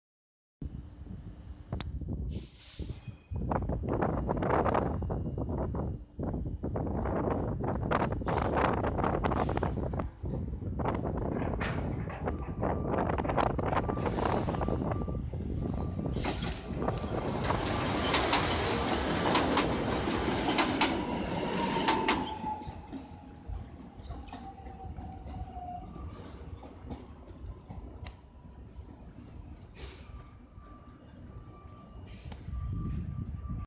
[KOLEJE ŚLĄSKIE] 36WEa-008 jako Os 97421 "S5" || Węgierska Górka
Impuls KŚ 36WEa-008 jako Os 97421 "S5" Katowice - Zwardoń wjeżdża na stację Węgierska Górka.